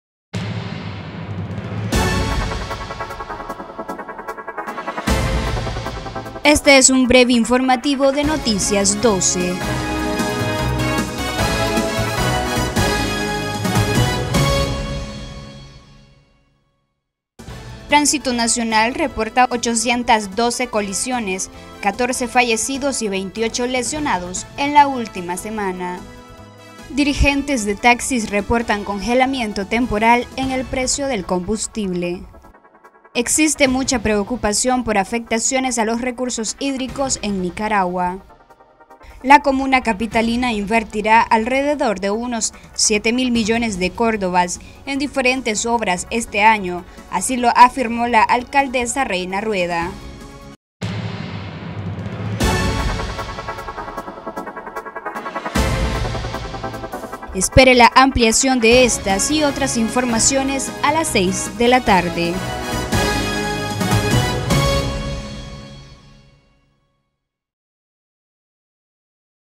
Breve Informativo
📍Tránsito reporta 812 accidentes y 14 fallecidos en la última semana
📍Dirigentes de taxis reportan congelamiento temporal de los combustibles
📍Existe mucha preocupación por afectaciones a los Recursos Hídricos en Nic.
📍ALMA invertirá más de C$7 mil millones en 2022